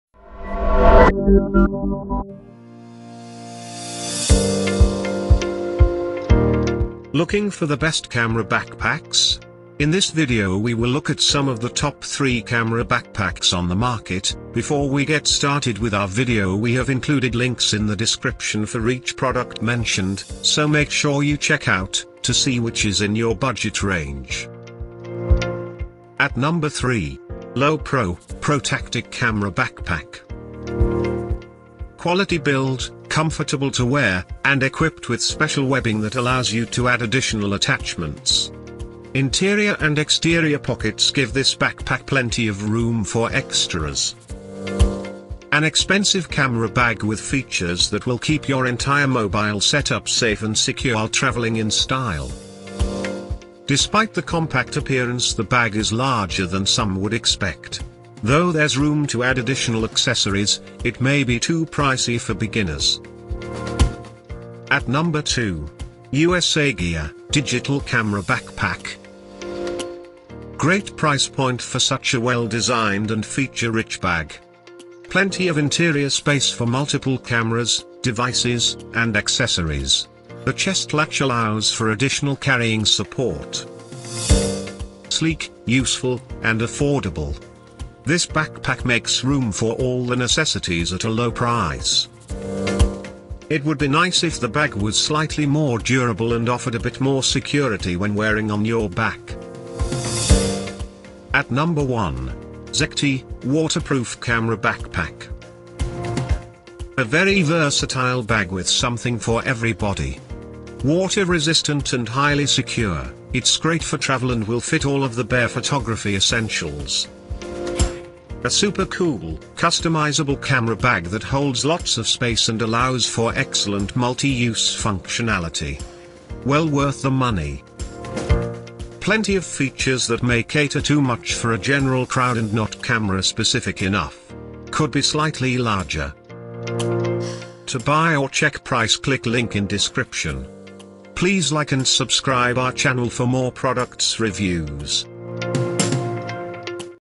Best Camera Backpacks, According To Customer Reviews in 2023
🌐 Best Camera Backpacks

1. Zecti, Waterproof Camera Backpack.
rpF1xg

2. USA Gear, Digital Camera Backpack.
sFN3

3. Lowepro, ProTactic Camera Backpack.
ELEt4

0:00 Top 3 Best Camera Backpacks
0:26 Lowepro, ProTactic Camera Backpack.
1:05 USA Gear, Digital Camera Backpack.
1:41 Zecti, Waterproof Camera Backpack.